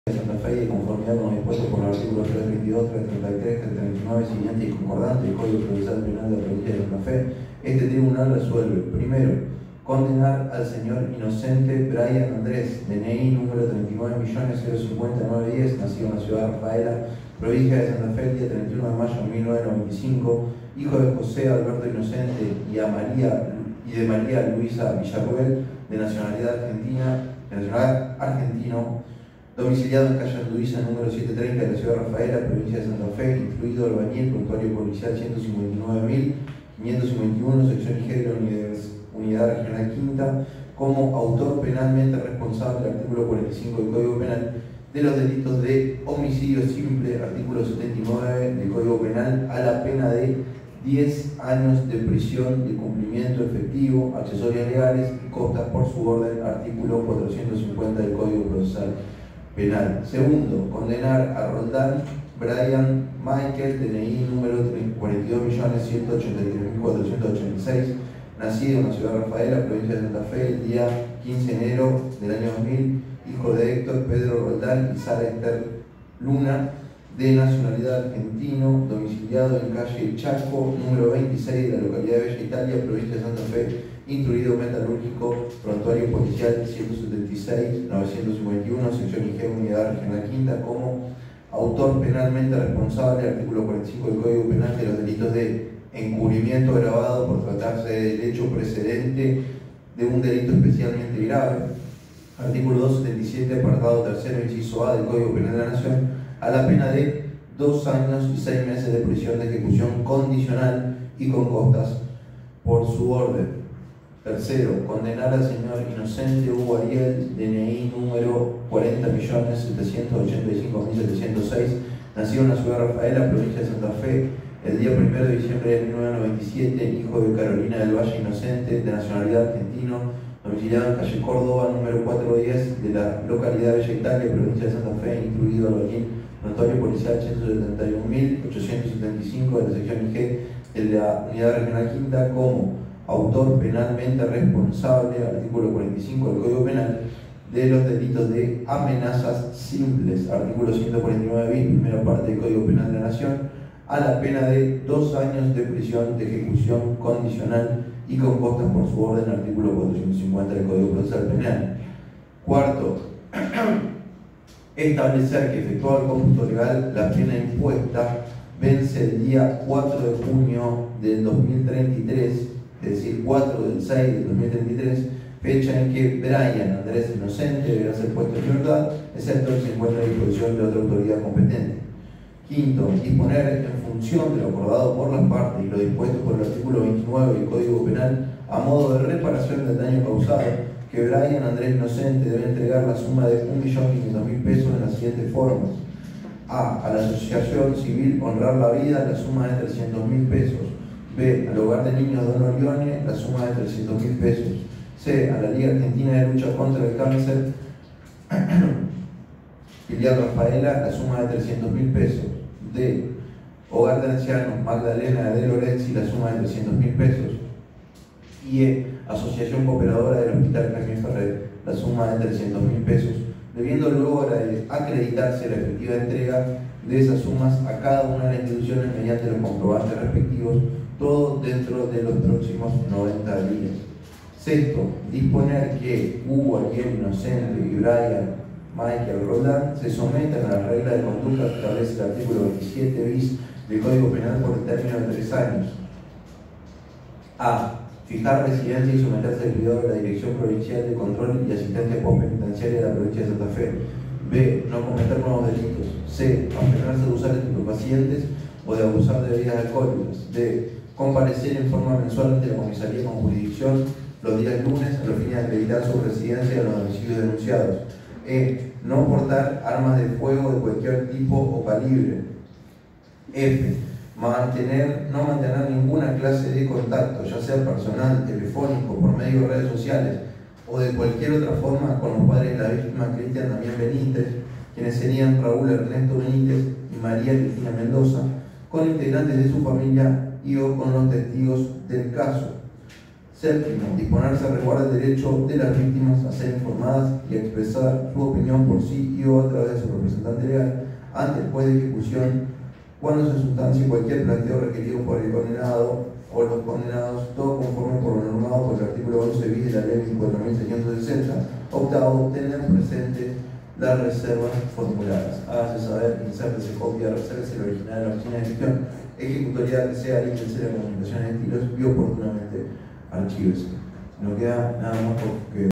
Lectura del fallo condenatorio por el crimen de Cristina Benítez
El suceso ocurrió el 4 de junio, en Alvear y Sarmiento. Brian Inocente fue hallado responsable del crimen y condenado tras un juicio de procedimiento abreviado.